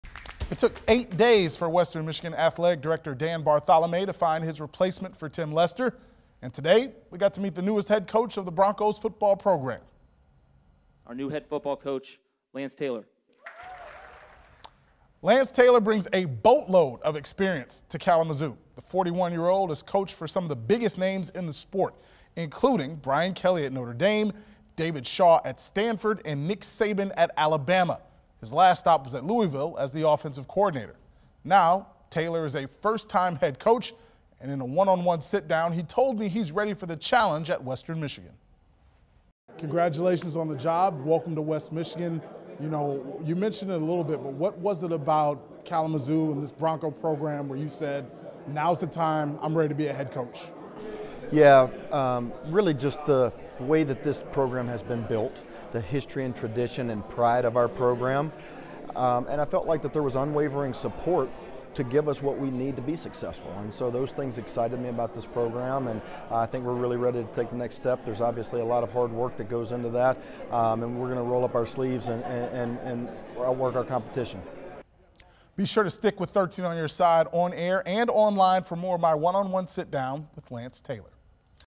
Lance Taylor introduced as new Western Michigan head football coach
The Western Michigan Broncos have found their new head football coach after the program let go of Tim Lester not so long ago.